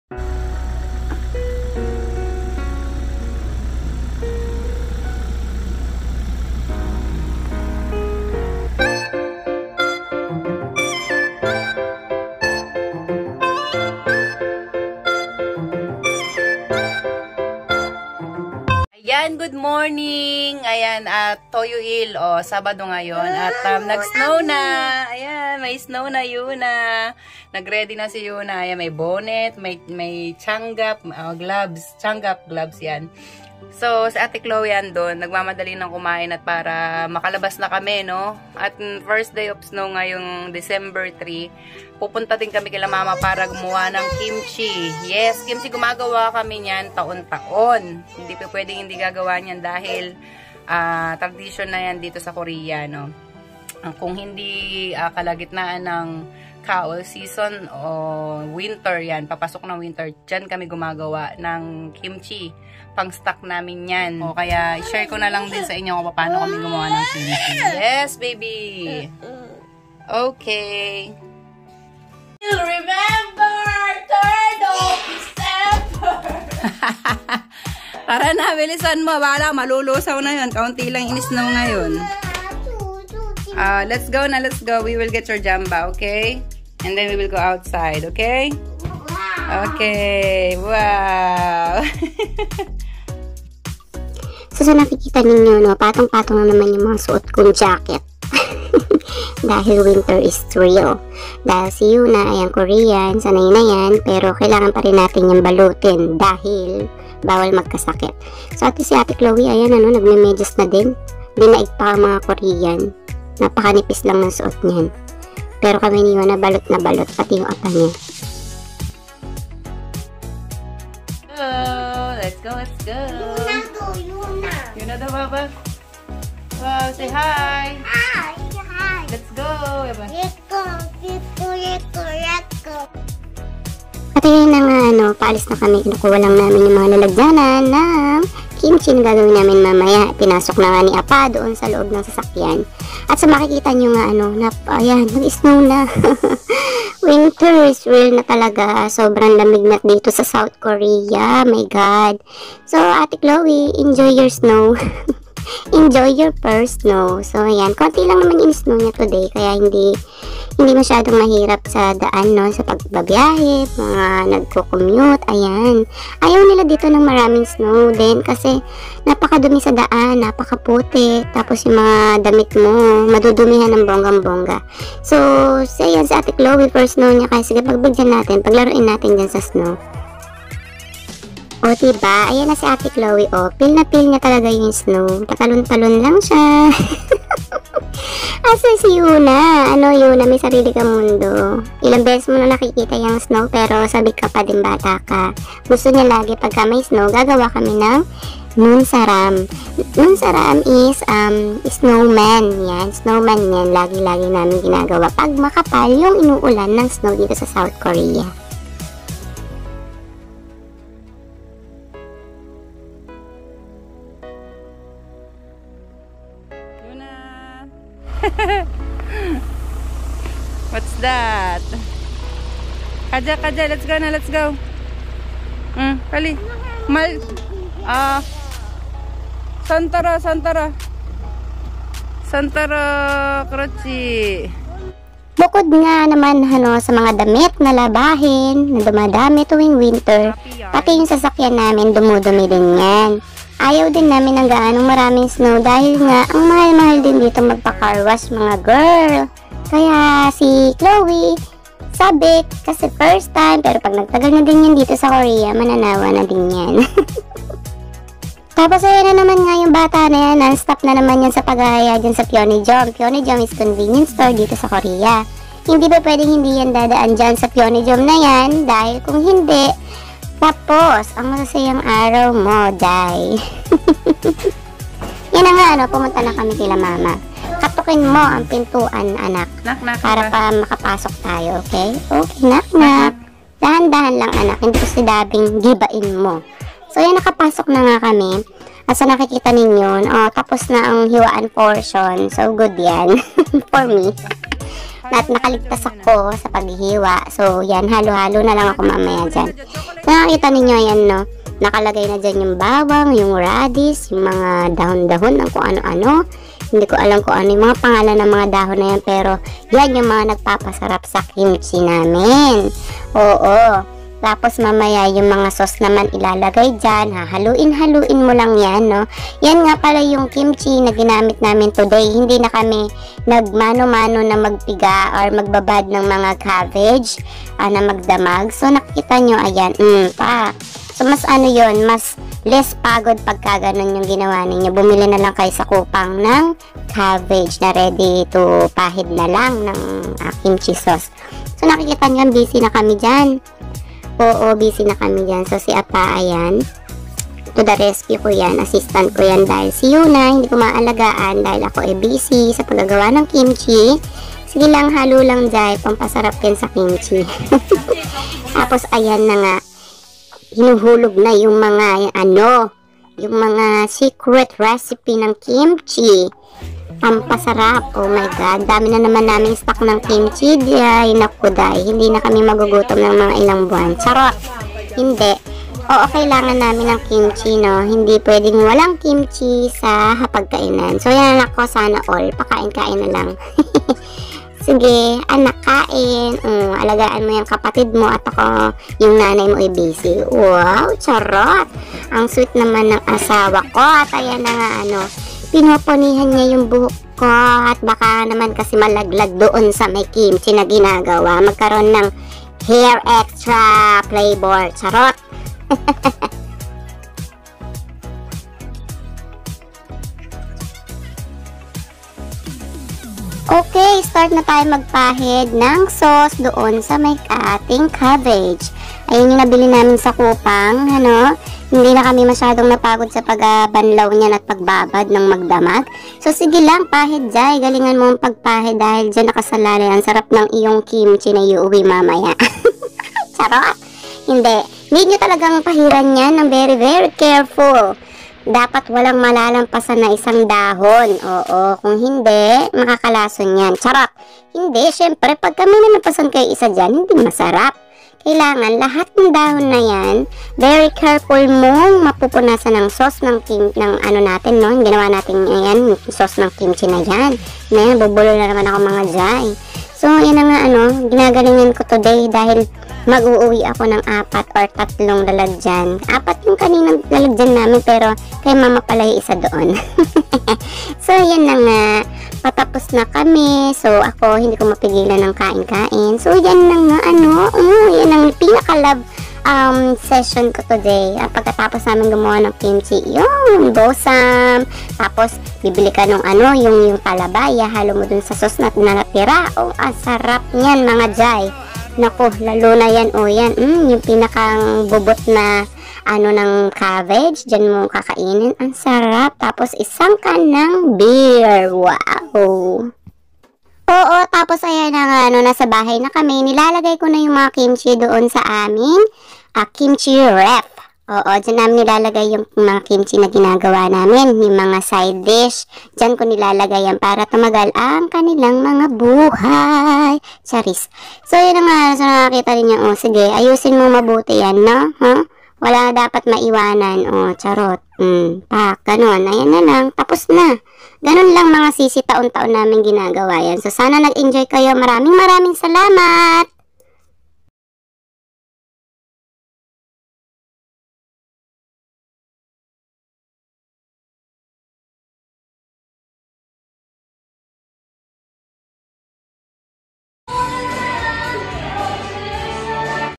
FirstSnow AteChloe'sFirstSnowExperience
Yung First Time Mo makakita ng Snow at gusto mong maenjoy,Kaso Schedule pala natin ng paggawa ng Kimchi😂

Hayaan mo Ate Chloe Madami pa namang next time,mas malakas pa jan at mas madame makakagawa tayo ng snow man🥰🥰🥰

at sigurado ako mag kakasundo kayo ni yuna pag dating sa paggawa ng snow❄️❄️❄️☃️☃️☃️